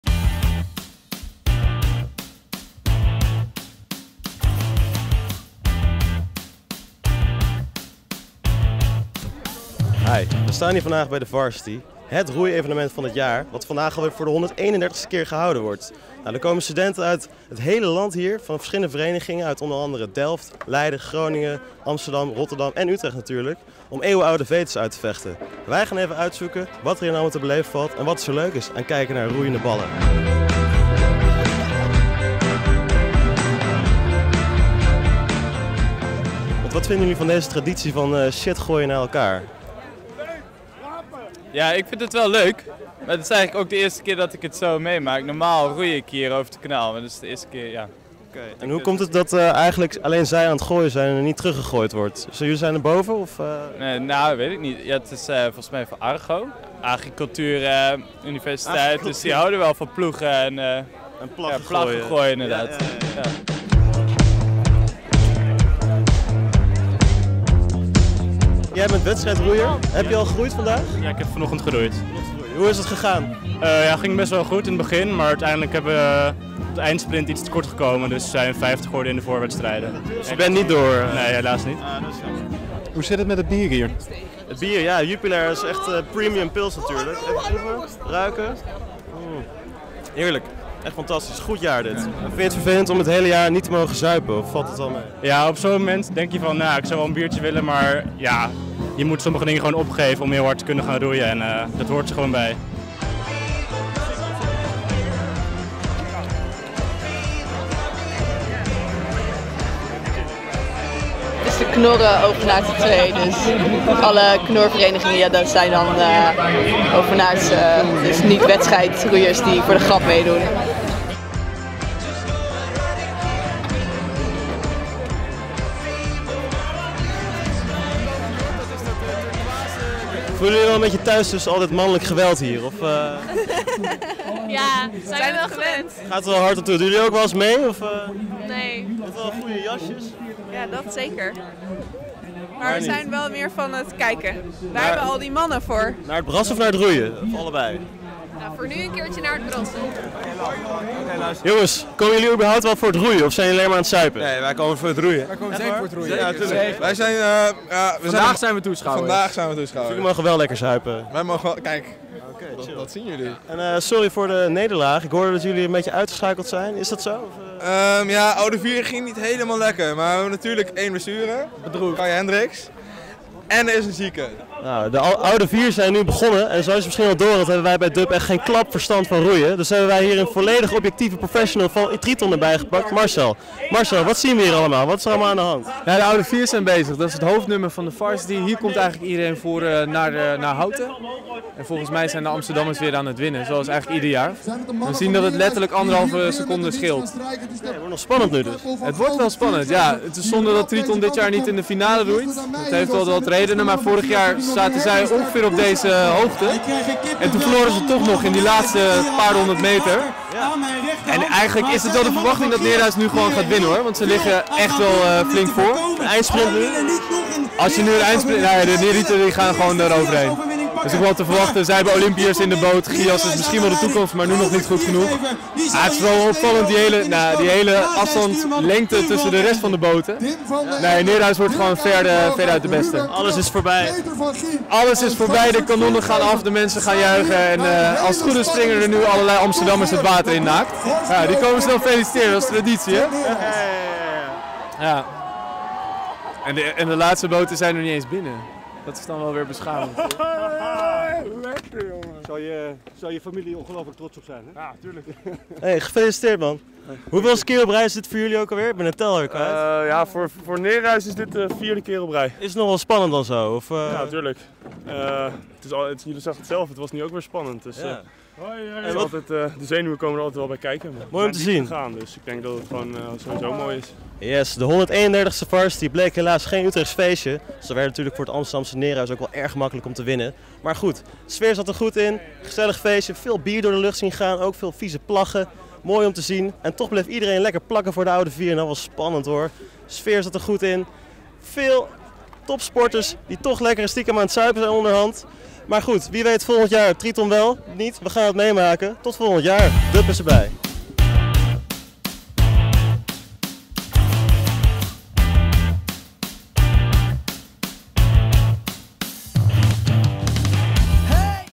DUB recenseert de Varsity
Afgelopen weekend vond de Varsity plaats, het jaarlijkse zuip- en roeifestijn waarbij Nereus altijd wint. DUB ging er heen om de sfeer én het bier te proeven.